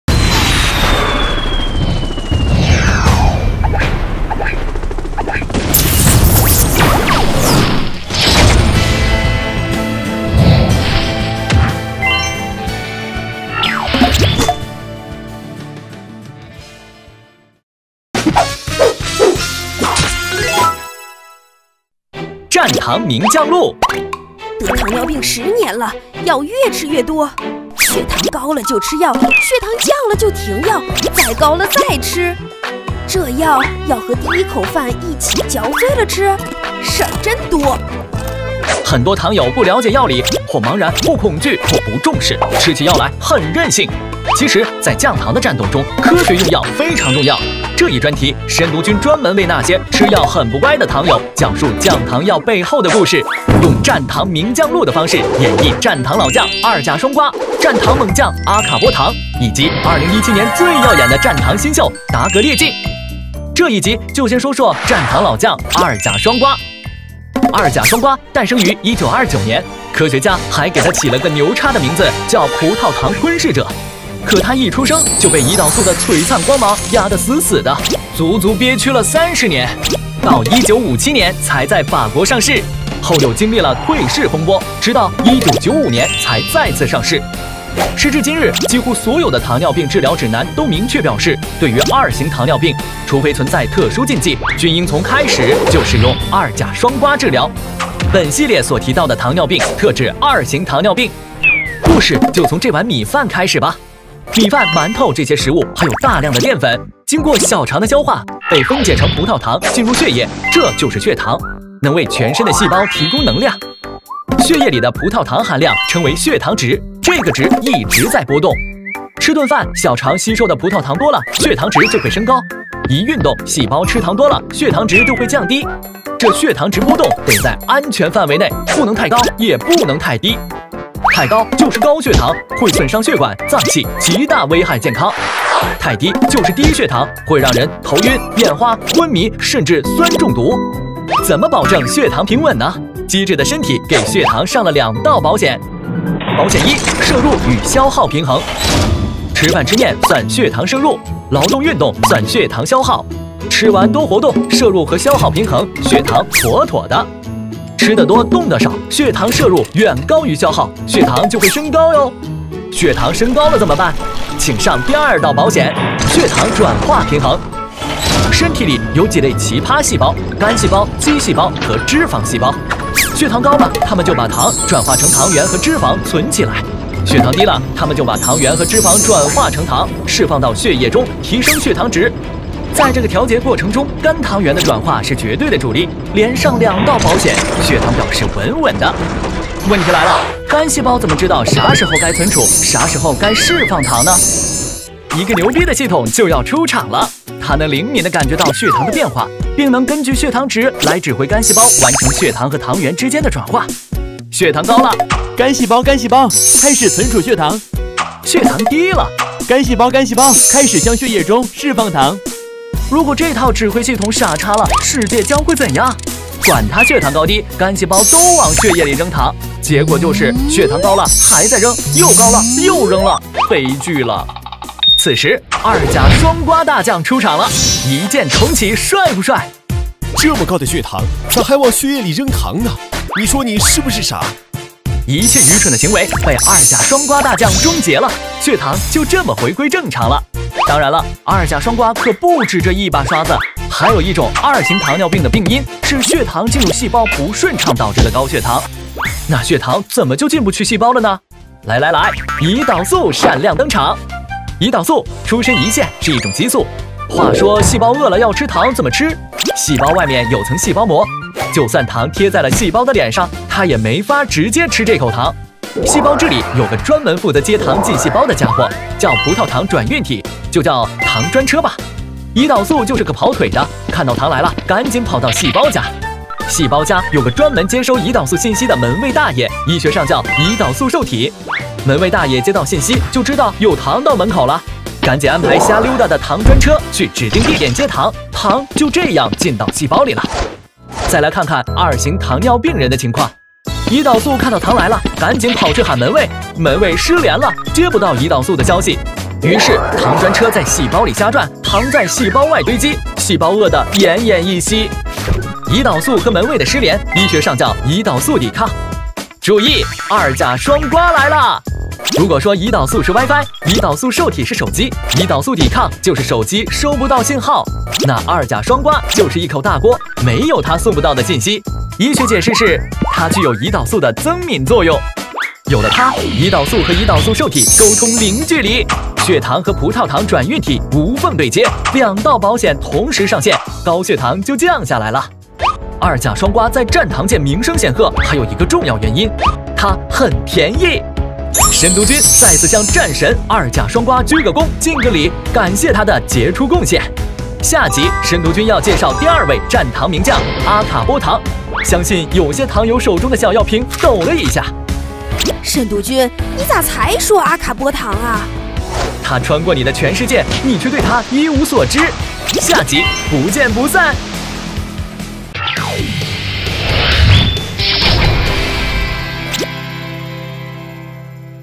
（繁體）降糖藥（01）100年來，2型糖尿病患者最大的福音居然是它？一種真正的神藥
二甲雙胍有何曆史？是2型糖尿病首選用藥？啥是血糖？攝入與消耗平衡、血糖轉化平衡如何調控血糖？肝糖原輸出增多會導致高血糖？二甲雙胍能抑制肝糖原分解？胰島素是啥？怎麽與胰島素受體起作用？葡萄糖轉運體如何把糖轉入細胞？啥叫胰島素抵抗？二甲雙胍有胰島素增敏作用？